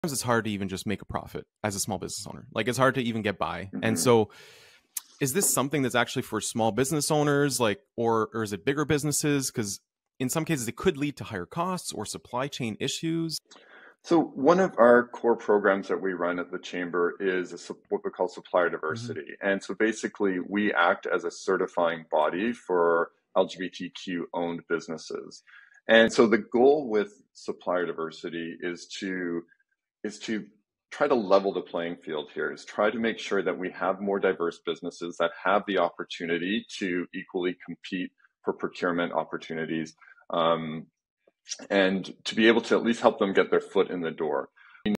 Boosting Small Business Diversity Overcoming Challenges and Driving Change
Darrell Schuurman (CGLCC) and Eric Saumure (Zenbooks)

Find the full episode of Small Business | Big Insights Podcast or with your podcast provider.